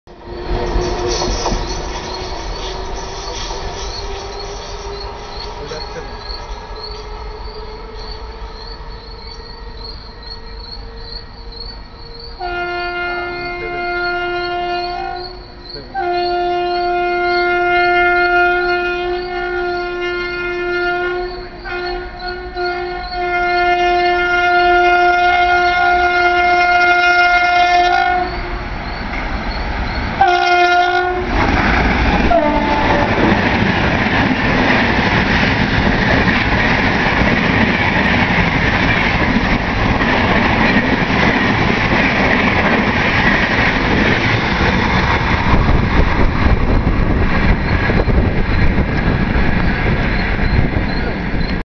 RAGING WAP7 WITH THE FASTEST DURONTO OF THE COUNTRY....SDAH DURONTO IN ALL HER MIGHT
WHITE STALLION OF INDIAN RAILWAYS aka HWH WAP7 30329 THUNDERS THROUGH THE SILENT BALARAMBATI STN OF HB CHORD WITH THE 3RD FASTEST TRN OF ER 12259 SDAH-NDLS DURONTO EXPRESS. VIDO DEDICATED TO MY FAV VIVIAN BOYE FOR HIS AWSOME CAPTURE